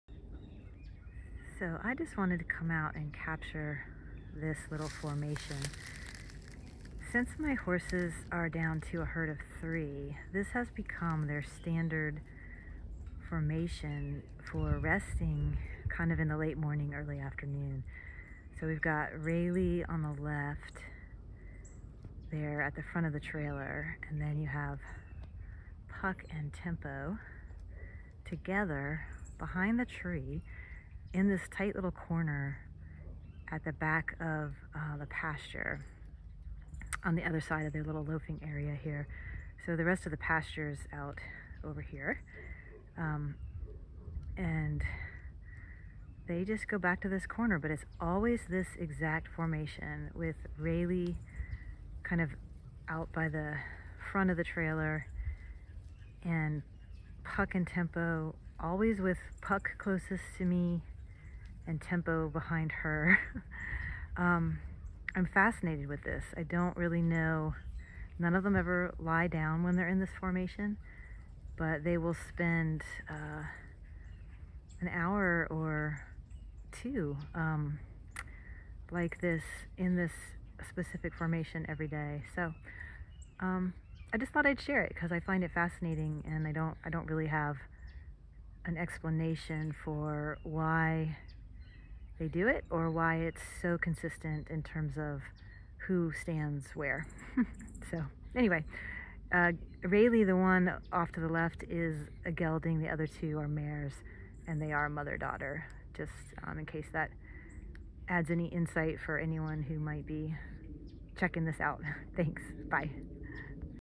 Interesting Resting Formation - Horses
This video shows a new "resting formation" my herd of three horses has adopted in the afternoons, about a year after the passing of one of their herd mates and two years after the passing of the herd elder. In terms of herd hierarchy (resource guarding), the mare closest to the camera is third, the gelding is second and the mare furthest from the camera holds the top spot.